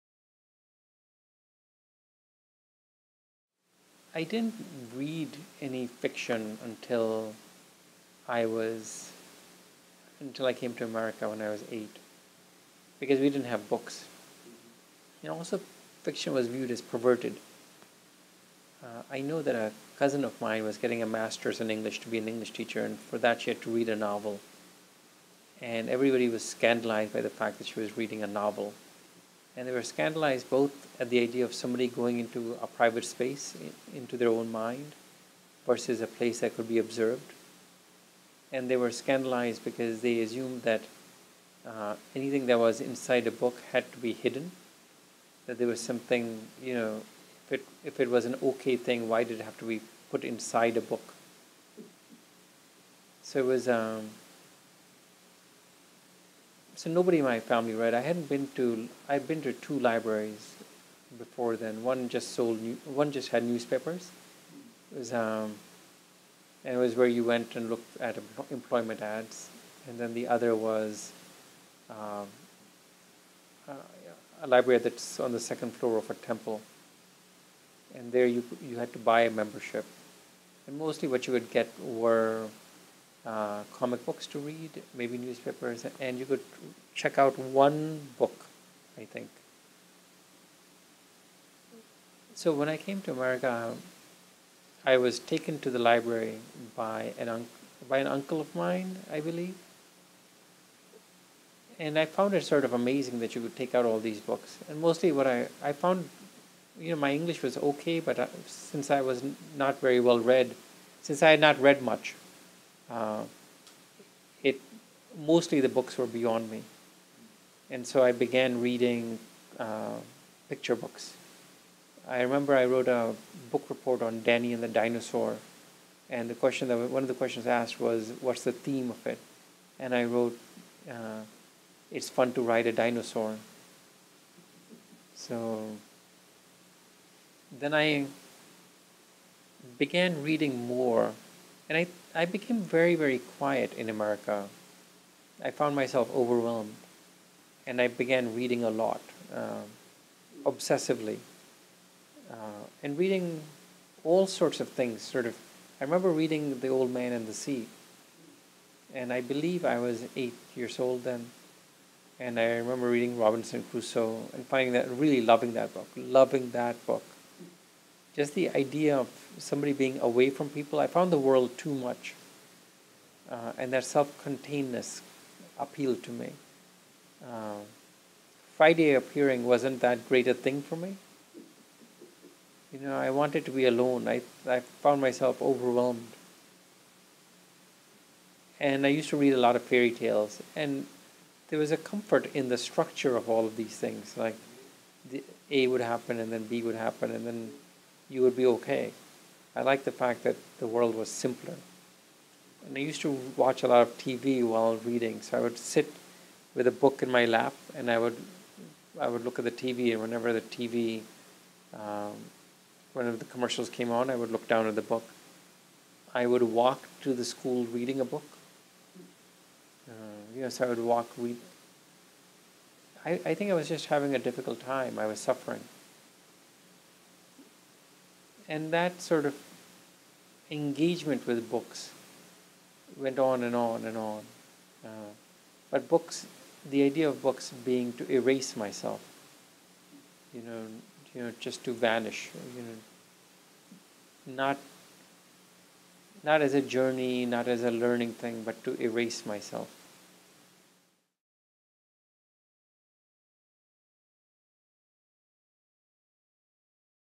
Akhil Sharma at the NYS Writers Institute in 2014
Akhil Sharma, "a supernova in the galaxy of young, talented Indian writers" (Publishers Weekly), received the PEN/Hemingway Award and the Whiting Writers' Award for his first novel, An Obedient Father (2000). His much-anticipated second novel is Family Life (2014), the story of Indian-American immigrants who are forced to cope after one of the family's two sons suffers a dreadful accident. Writing in advance praise, author David Sedaris called it, "Outstanding," and said, "Every page is alive and surprising, proof of [Sharma's] huge and unique talent."